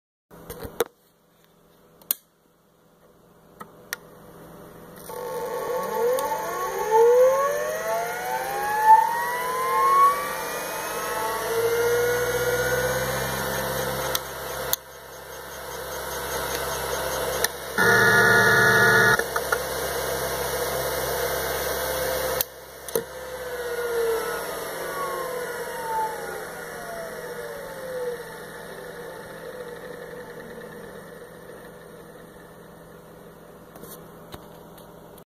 AT&T 97544SA Spinup/Spindown
Capacity: 327MB
Interface: Parallel SCSI
Spindle Speed: 3600 RPM(?)
Date: January ?, 1992